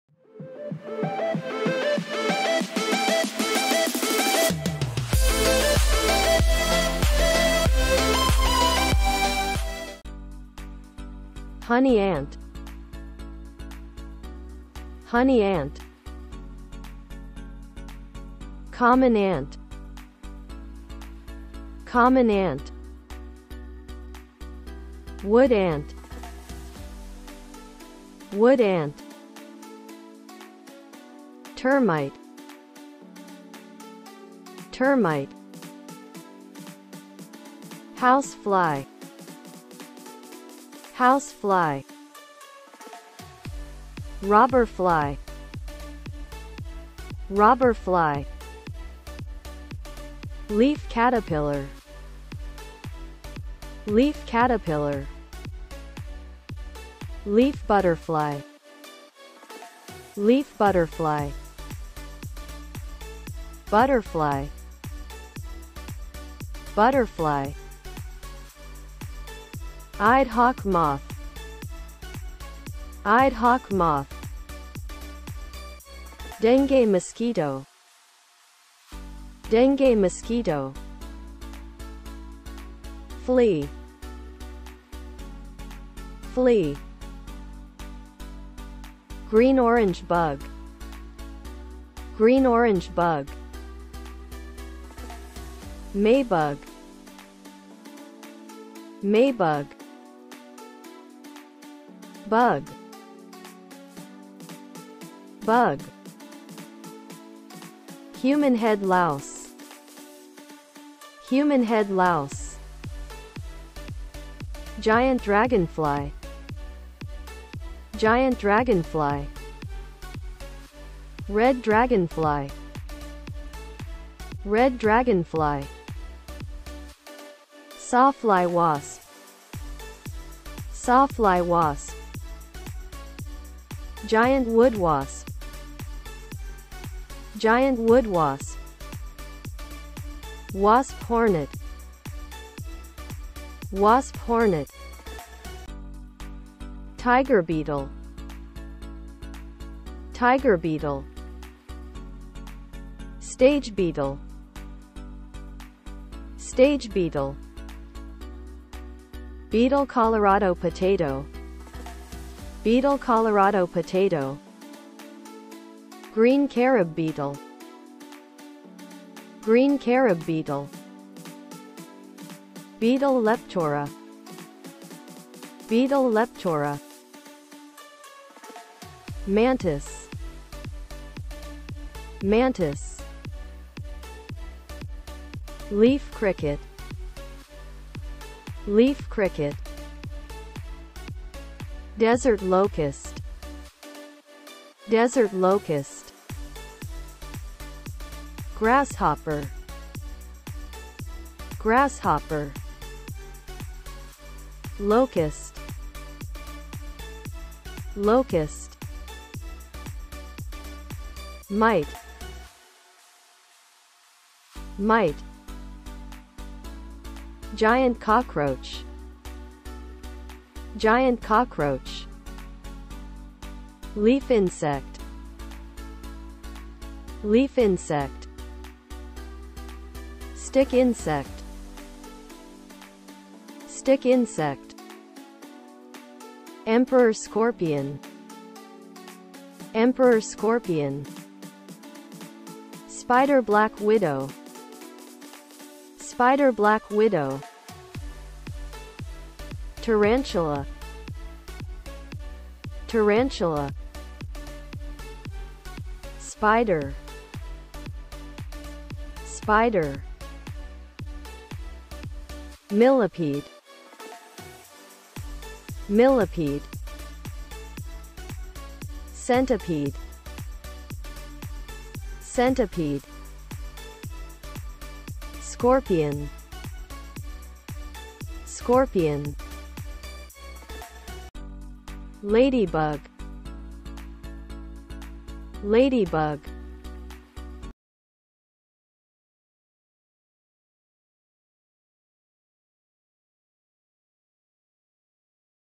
insects in english | with picture & pronunciation
In this video( insects in english) you will know the kind of insects with beautiful picture and american pronunciation